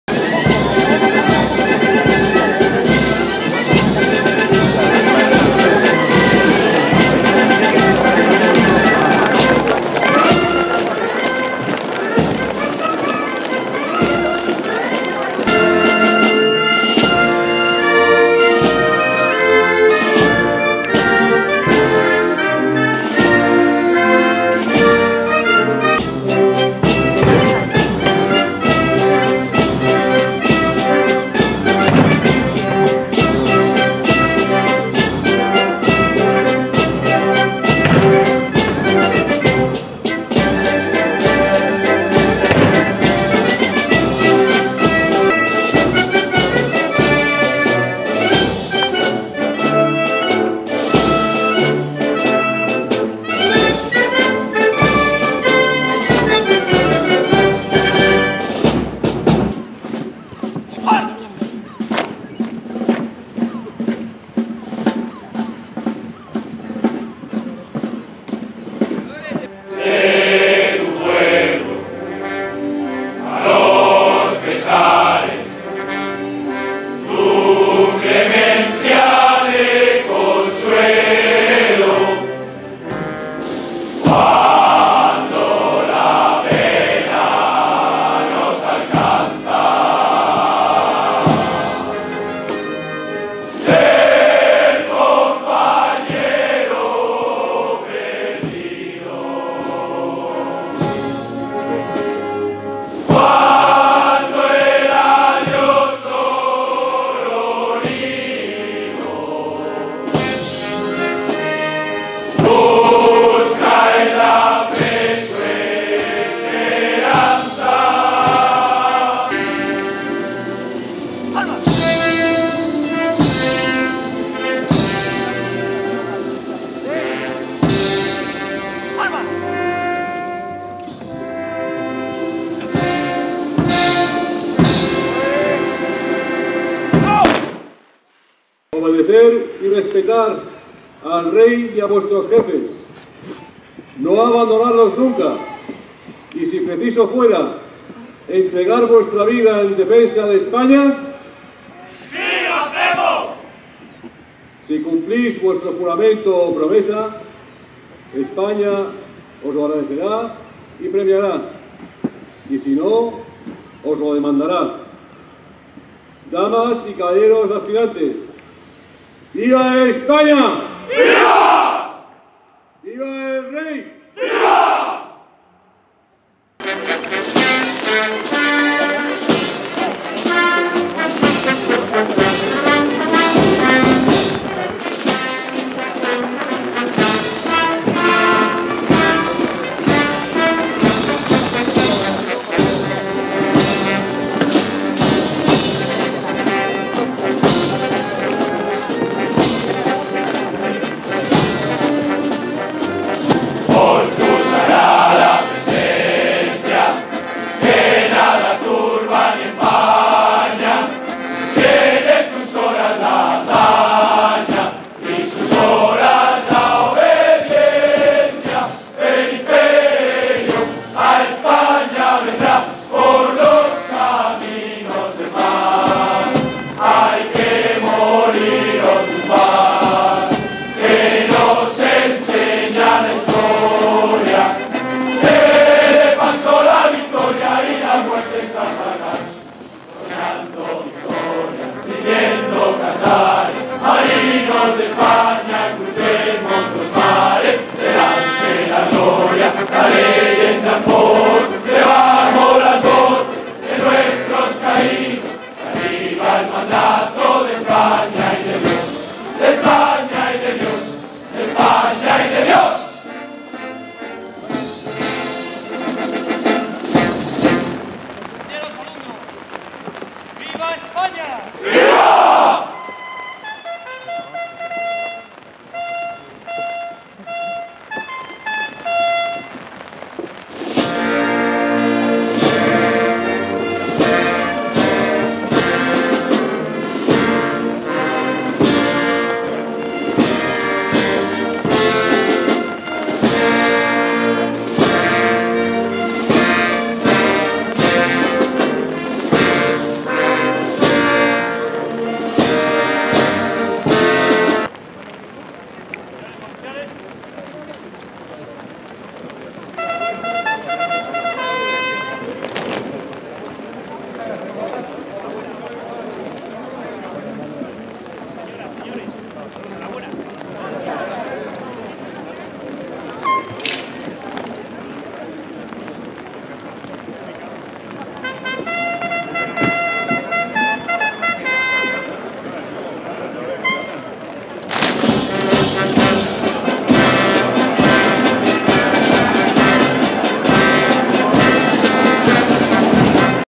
Jura de Bandera en la Escuela Naval Militar de Marín
Jura de Bandera en la Escuela Naval Militar de Marín el 28 de noviembre de 2014. Juraron los alumnos de intendencia e ingenieros de la Armada Española, y también personal civil, entre este yo.